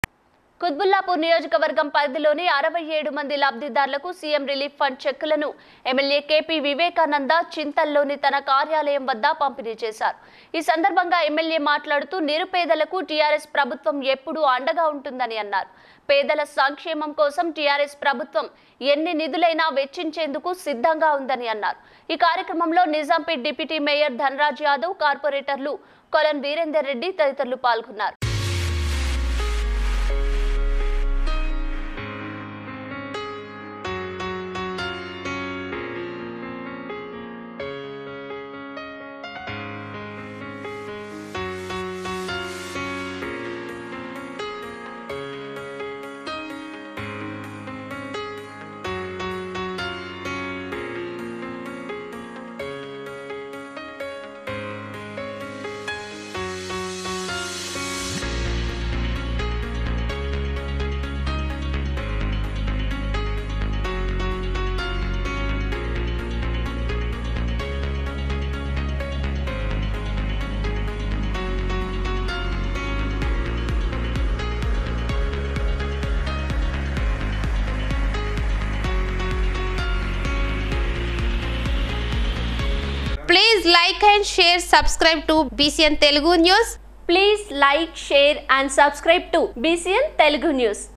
67 మంది లబ్ధిదారులకు సీఎం రిలీఫ్ ఫండ్ చెక్కుల పంపిణీ || Kutbullapur
CM KP Vivekanand MLA distributed relief fund checks to 67 beneficiaries in Kutch constituency at his office in Chintal. Speaking on the occasion, the MLA said that the TRS government will always be supportive of the poor. He said that the TRS government is ready to spend any amount of funds for the welfare of the poor. The event was attended by Nizampet Deputy Mayor Dan Raj Yadav, corporators Kolan Virender Reddy and others.
          కుత్బుల్లాపూర్ నియోజకవర్గం పరిధిలోని 67 మంది లబ్ధిదారులకు సీఎం రిలీఫ్ ఫండ్ చెక్కులను ఎమ్మెల్యే కేపి వివేకానంద్  చింతల్ లోని తన కార్యాలయం వద్ద పంపిణీ చేశారు. ఈ సందర్భంగా ఎమ్మెల్యే  మాట్లాడుతూ నిరుపేదలకు టిఆర్ఎస్ ప్రభుత్వం ఎప్పుడూ అండగా ఉంటుందని అన్నారు.. పేదల సంక్షేమం కోసం టిఆర్ఎస్ ప్రభుత్వం ఎన్ని నిధులైనా వెచ్చించేందుకు సిద్ధంగా ఉందని అన్నారు.  ఈ కార్యక్రమంలో నిజాంపేట్ డిప్యూటీ మేయర్ దన్ రాజ్ యాదవ్, కార్పొరేటర్లు కొలన్ వీరేందర్ రెడ్డి తదితరులు పాల్గొన్నారు.